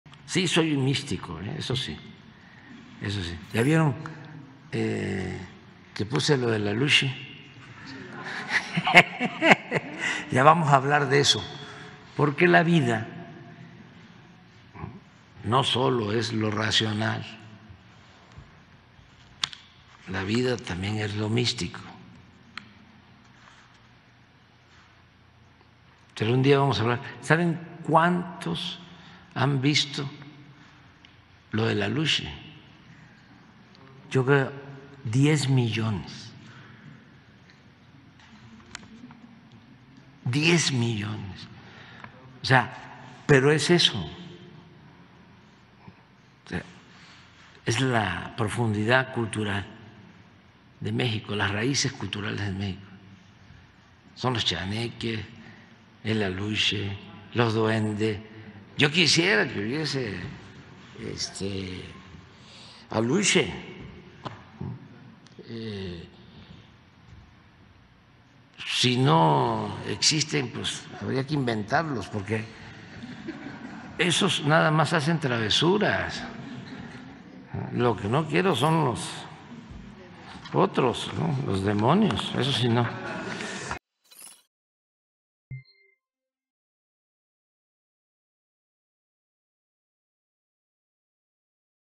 Si los aluxes no existen, habría que inventarlos, dice AMLO al presumir 10 millones de views
El presidente Andrés Manuel López Obrador presumió que su foto en Twitter sobre la presunta aparición de un aluxe en la construcción del Tren Maya ha sido visto por 10 millones de personas